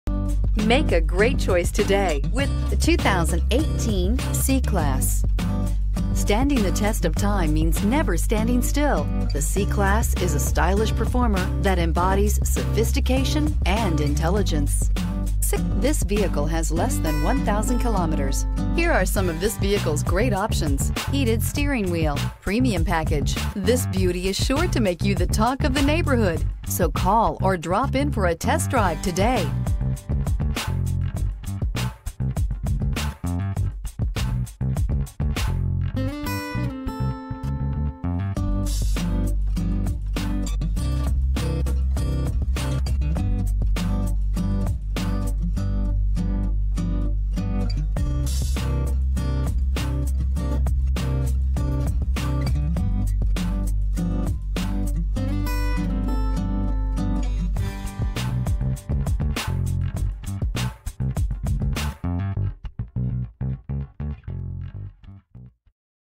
2018 Mercedes-Benz C300 Brampton ON 18MB727
2018 Mercedes-Benz C300 4MATIC Coupe

Mercedes-Benz Brampton
10 Coachworks Crescent

Looking for the right 2-door coupe? Today could be your lucky day. this 2018 Mercedes-Benz C300 could be yours.